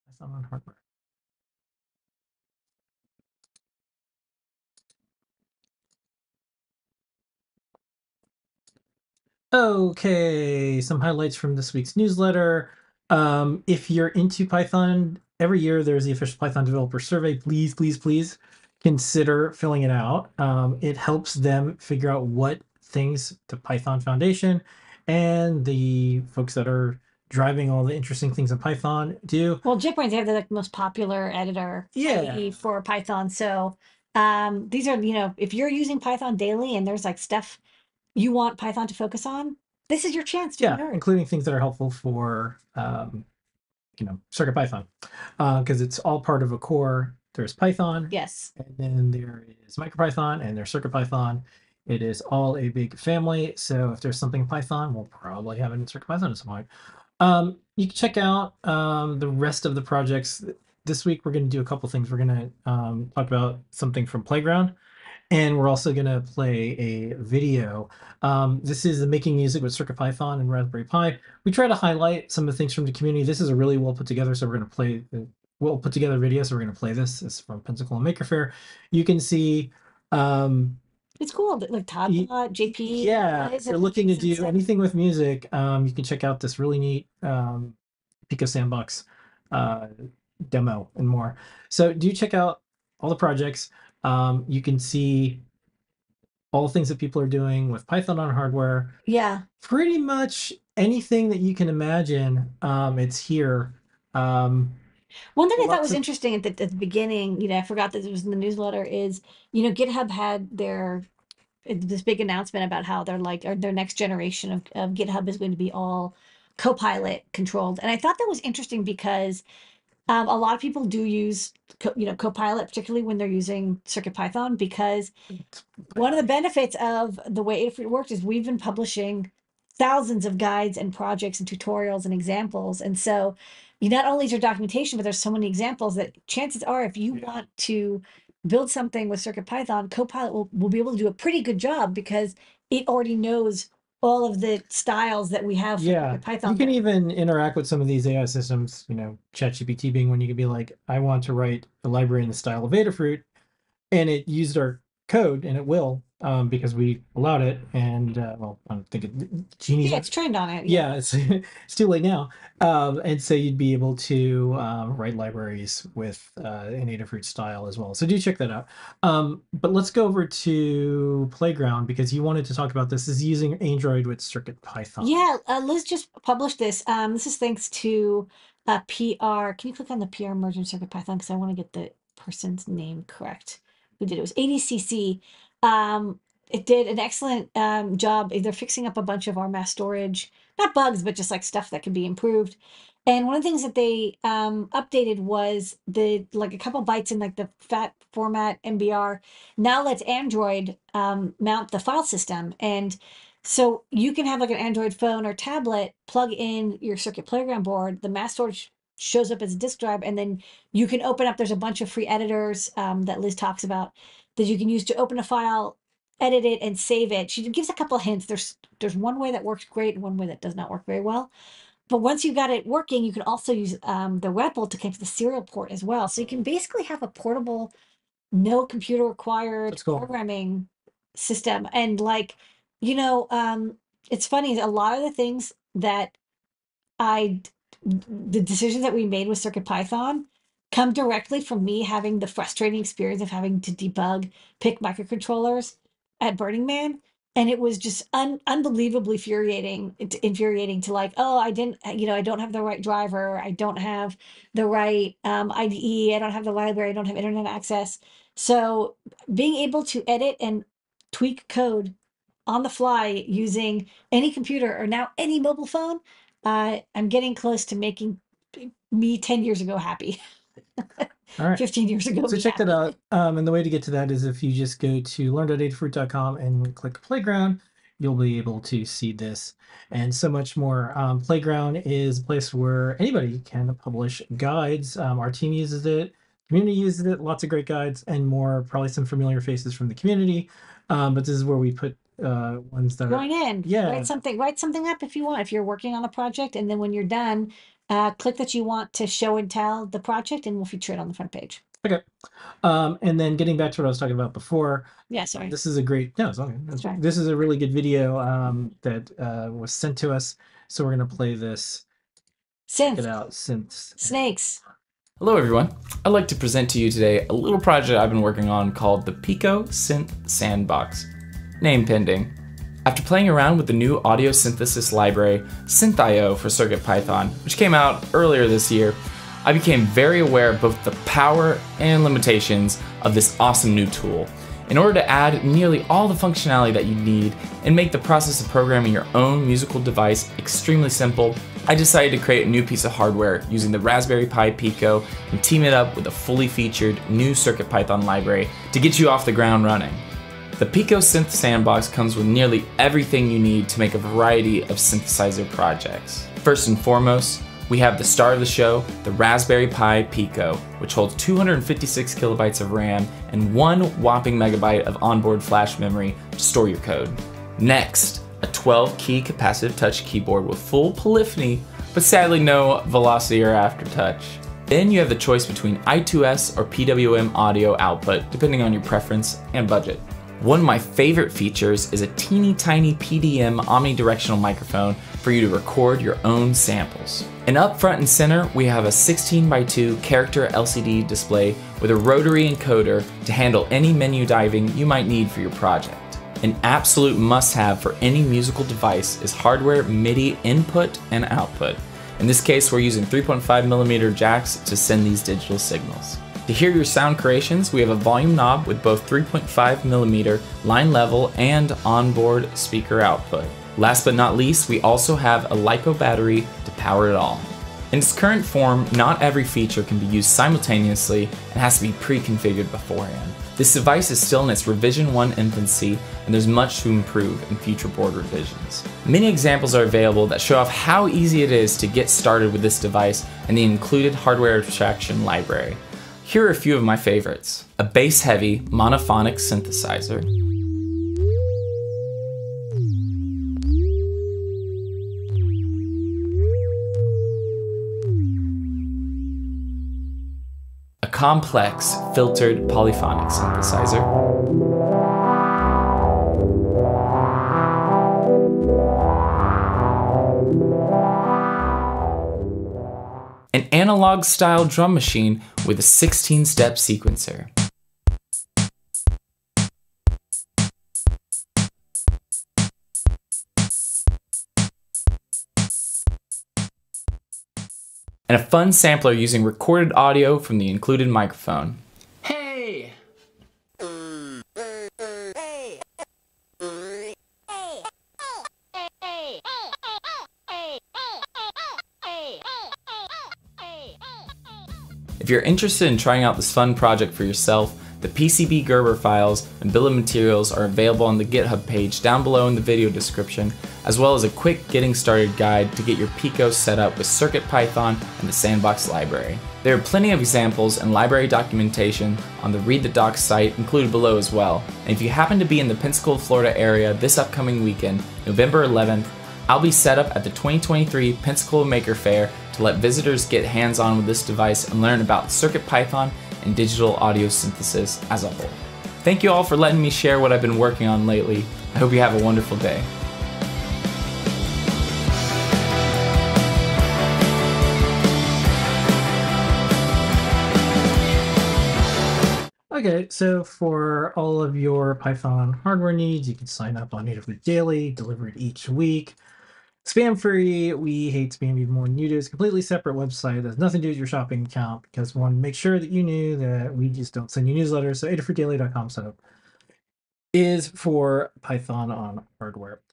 Python on Hardware weekly video Nov 15, 2023
The wonderful world of Python on hardware! Episode 255 (November 15, 2023). This is our weekly Python video highlights! Ladyada and PT review the Python on hardware news of the week.